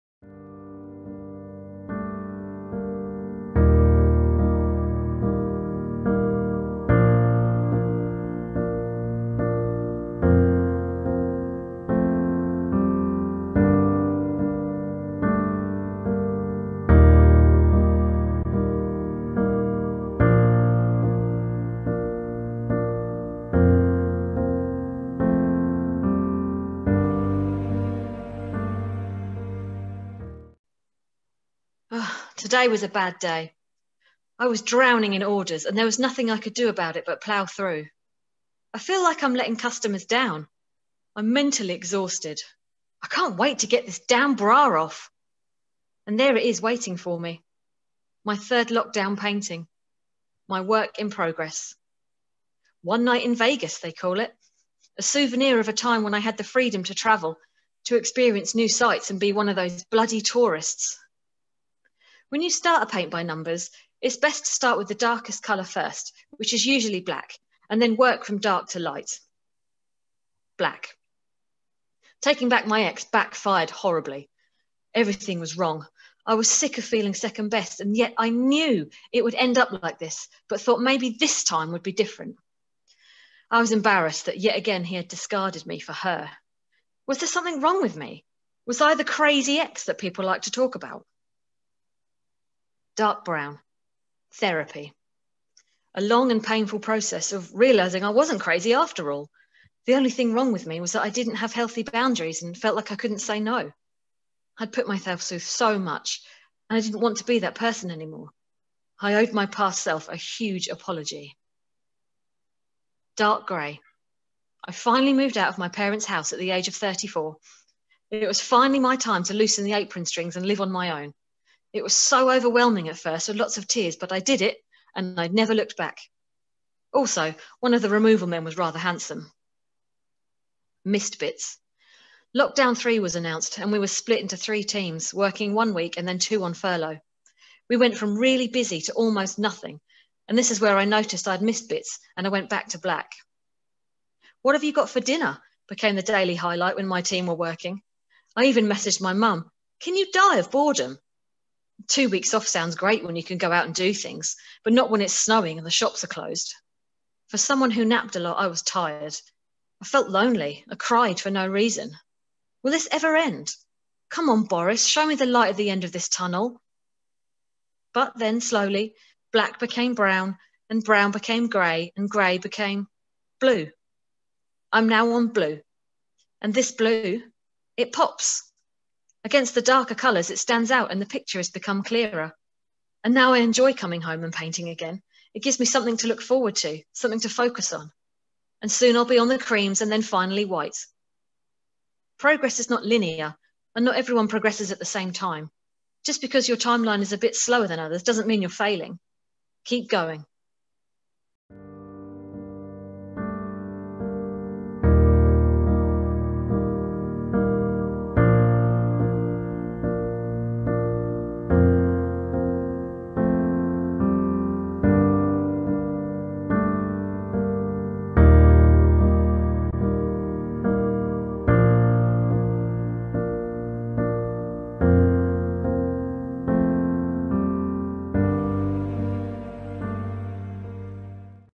Work in progress by Kitty Whitley
A short story from our "Every picture tells a story" project where members of our Black Dog Theatre Creation have been working with Professional Photographer James Taylor exploring how pictures can be great stimuli for creating stories.

"Work in progress" is written and performed by Kitty Whitley. It tells a sweet and poignant reflection upon the struggles in life during the Covid lockdown.